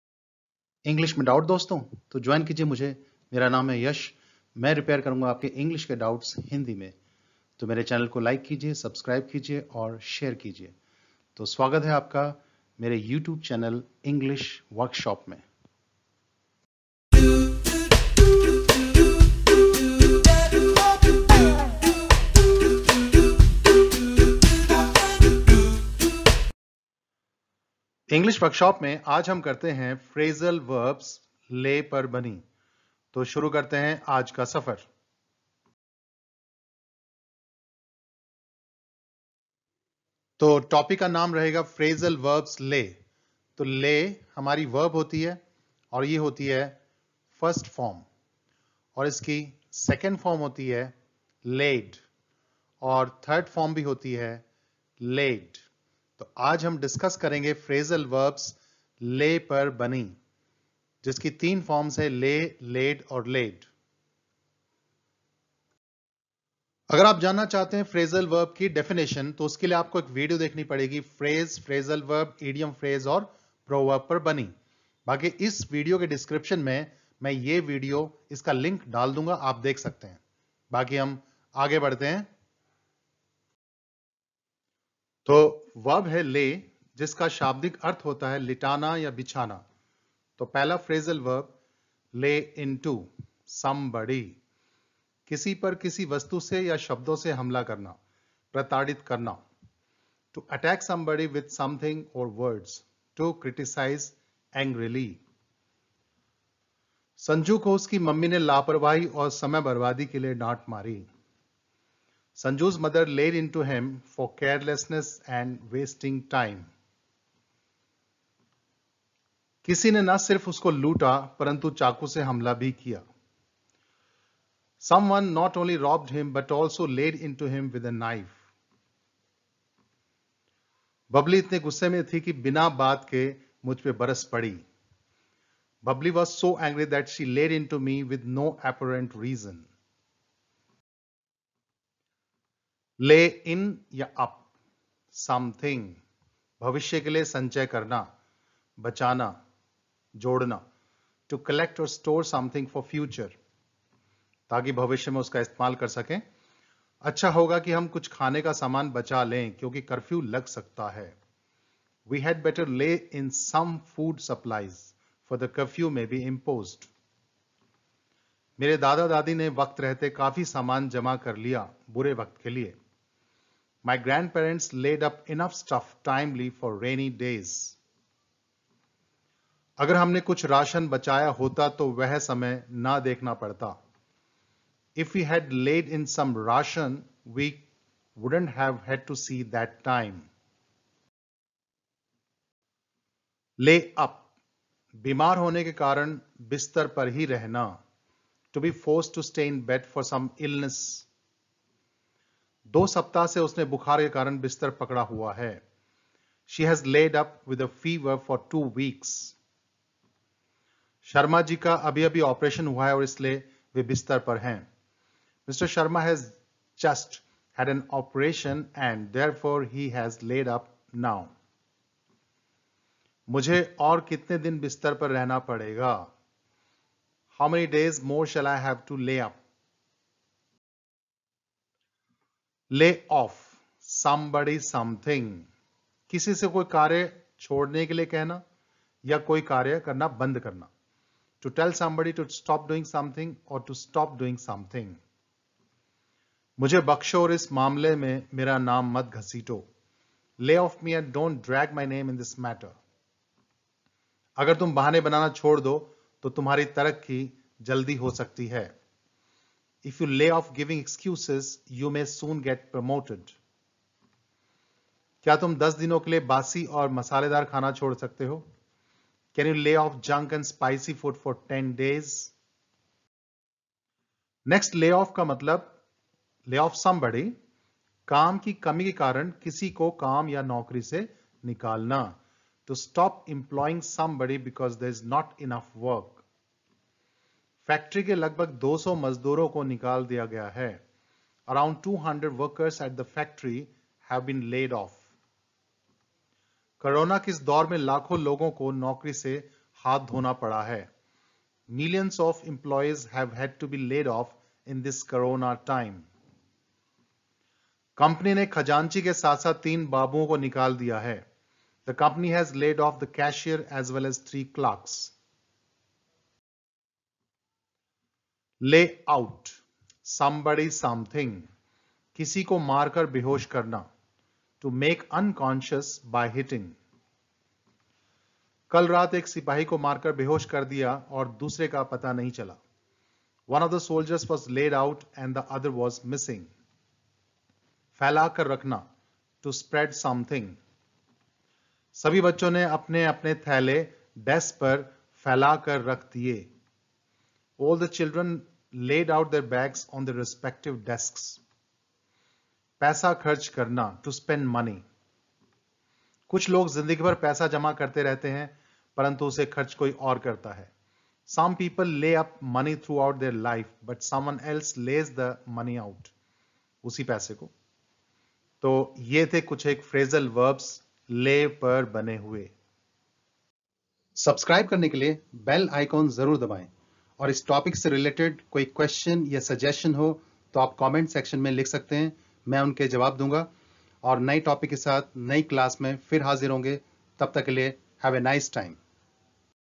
Phrasal Verbs: on Lay: Explanation/Examples/Translation/ English Grammar. (English Workshop) by Yash
Hello Friends
I am Yash with 20-year-teaching experience. In this video I have taken up Phrasal Verbs on the verb lay with explanation and examples in detail. Each example sentence has been translated into the Hindi language in order to enable the students to understand the point well. The video is very useful for all kinds of learners from different walks of life. If you wish to use English in speaking, writing, teaching or cracking any sort of competitive exams, the lecture in the video is a perfect choice for you. If you have any doubt or question pertaining to the topic, you may please drop it in the comment section. I will reply to it. I hope you enjoy the video. Wish you all the best!